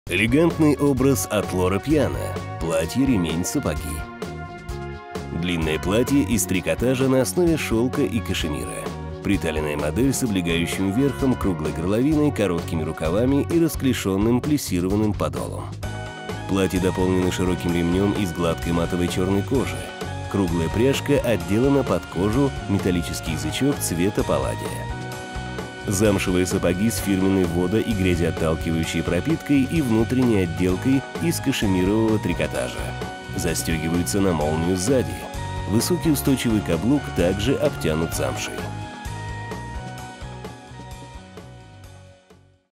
Как выглядеть стильно осенью? | Коллекция ОСЕНЬ-ЗИМА 2020/2021
Элегантный образ на каждый день. Утонченность, трендовые силуэты и эксклюзивные материалы в новой коллекции от от Loro Piana🤍🌾